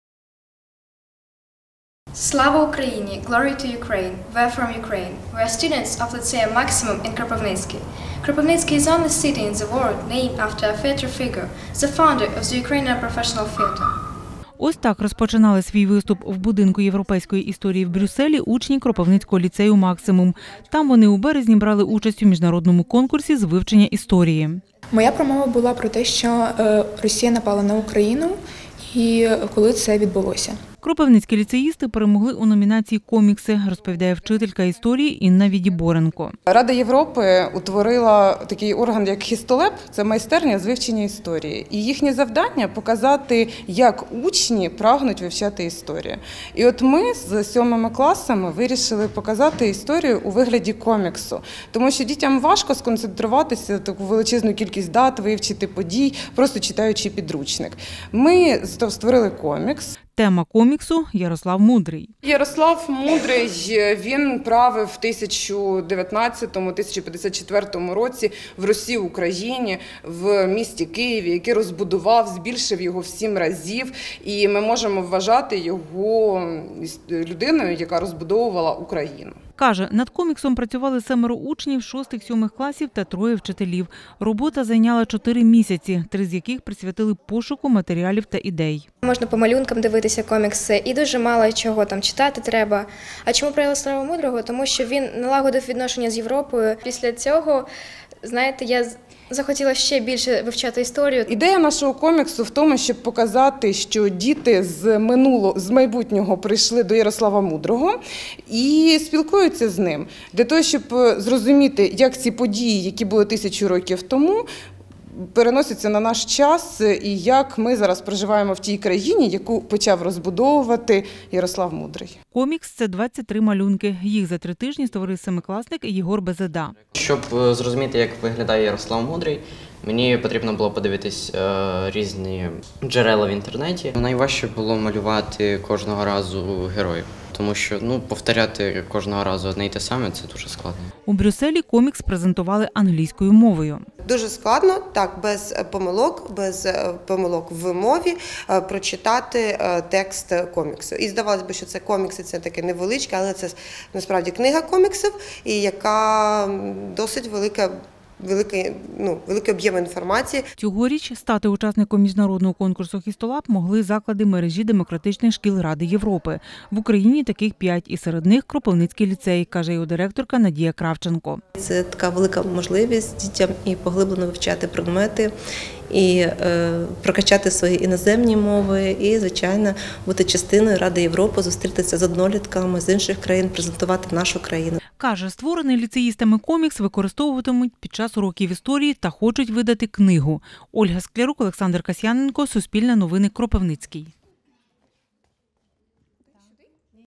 Кропивницькі учні перемогли в міжнародному конкурсі з коміксом про Ярослава Мудрого
"Він налагодив зв'язки з Європою". Кропивницькі учні перемогли в міжнародному конкурсі з коміксом про Ярослава Мудрого